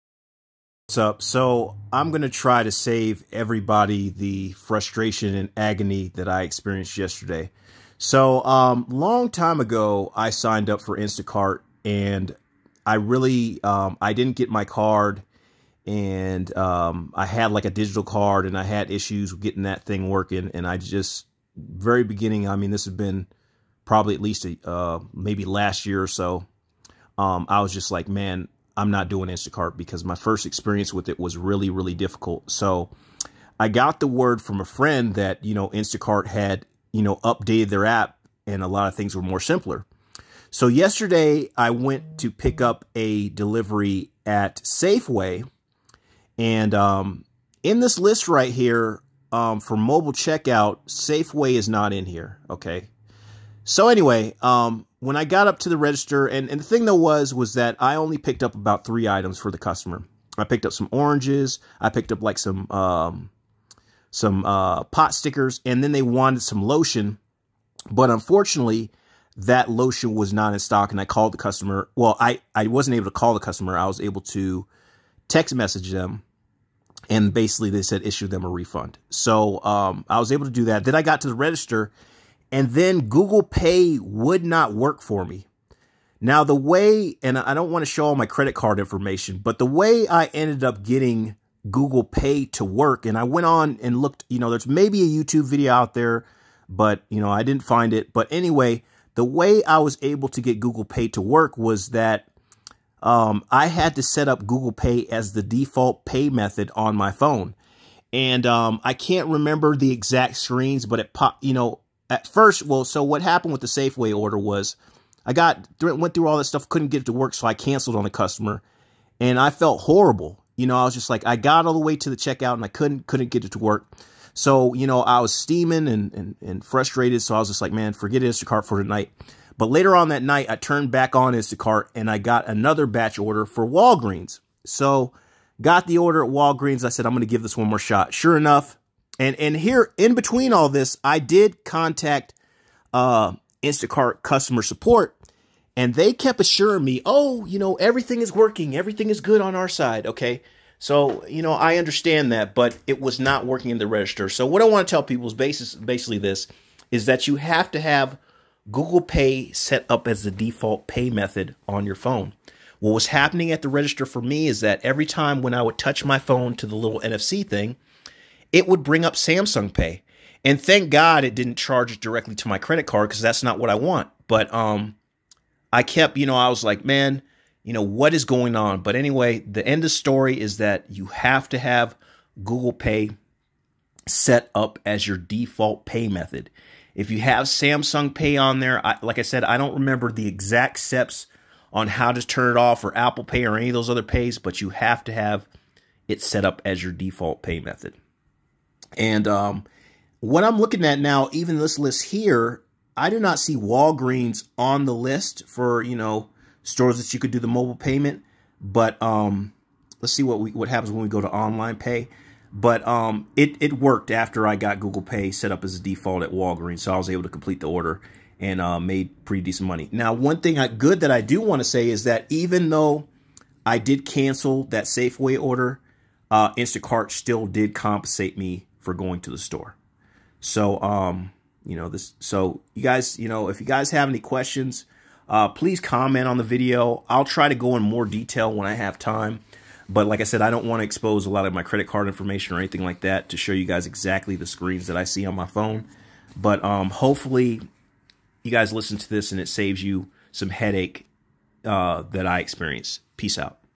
Google Pay must be set as the default payment method.
Customer support really didn't help me that much they just assumed that the card was working. It was really frustrating to try to get them to understand that my card kept going to Samsung Pay as a default payment method.